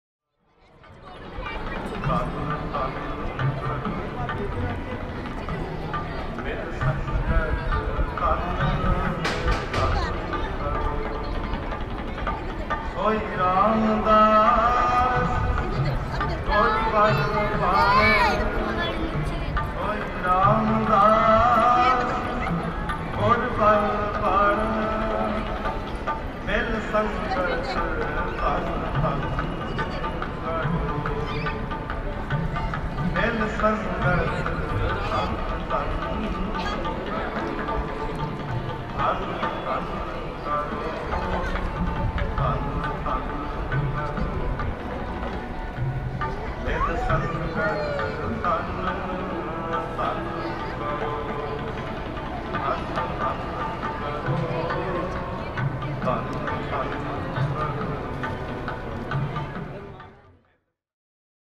📽 360° Cinemagraphs: Darbar Sahib | Amritsar, India 🇮🇳
A 360° GoPro VR Cinemagraph from inside the holy grounds of the Golden Temple - otherwise known at Darbar Sahib - the spiritual totem for the world's Sikhs, based in the Pubjabi city of Amritsar, India! =]  🇮🇳